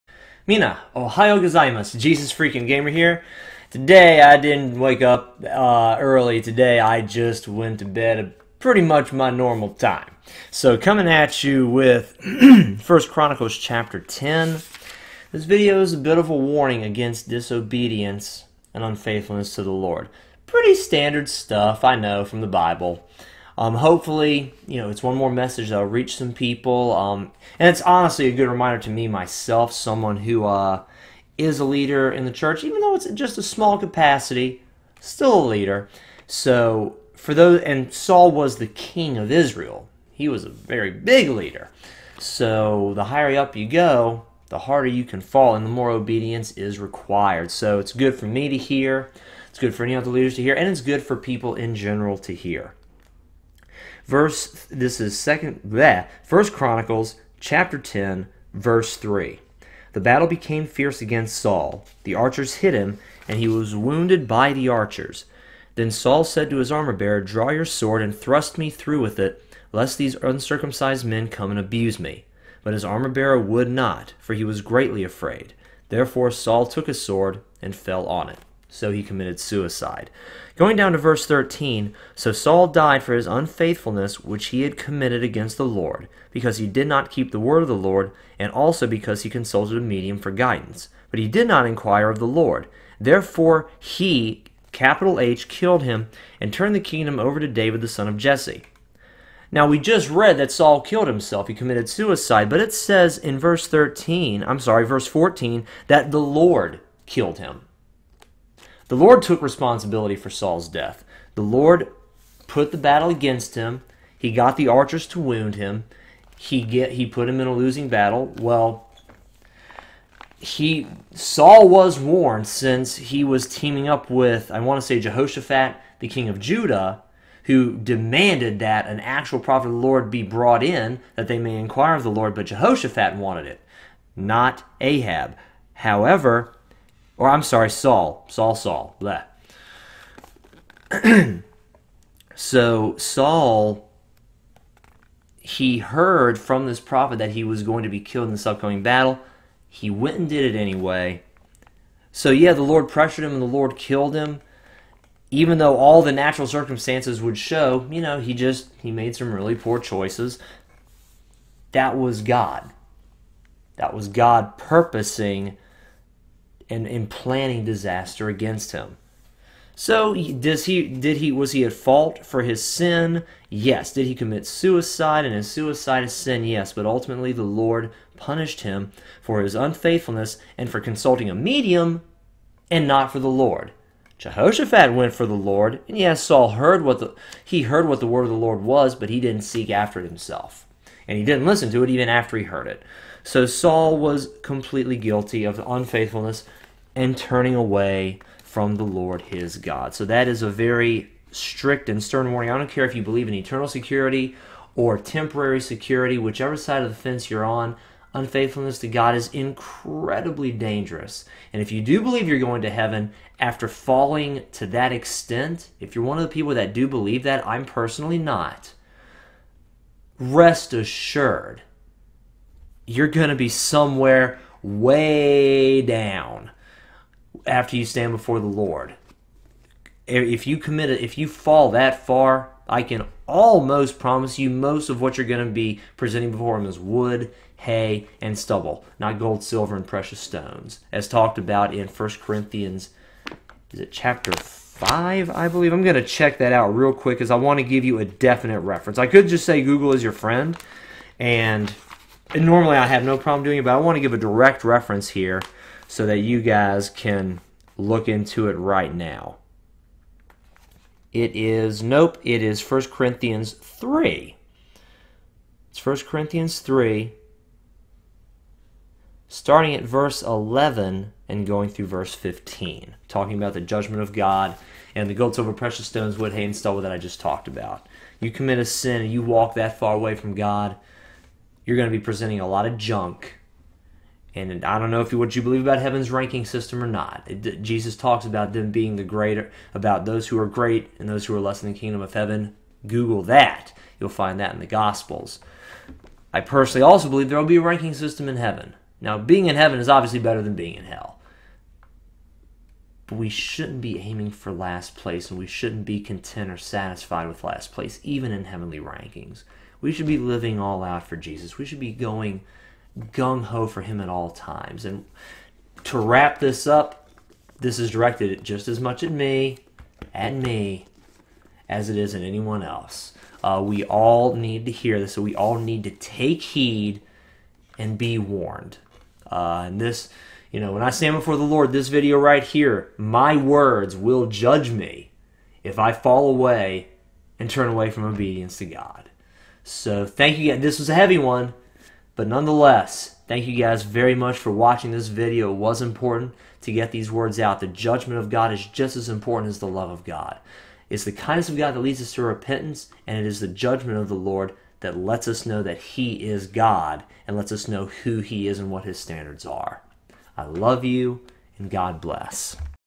A WARNING ABOUT DISOBEDIENCE AND UNFAITHFULNESS TO THE LORD
If you're thinking about giving up and doing that sin you love so much, or hard times have fallen and you're thinking about walking away from the Lord...DON'T!